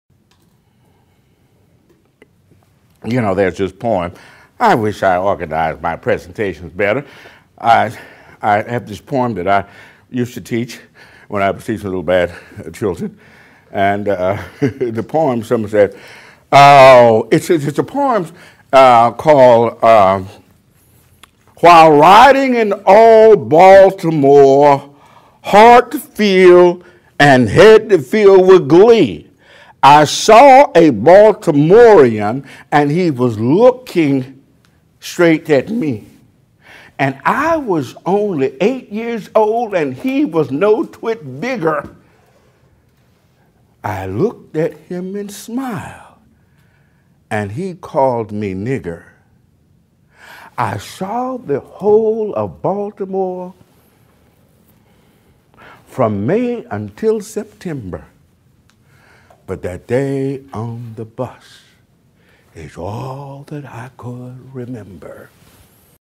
Jenkins On Diveristy at SLU-Baltimore Trip Poem
William Jenkins lectures at Saint Louis University in St. Louis on Diversity.  His engaging presentation was filled with discussion, laughter, challenges and poetry. Here's a short clip.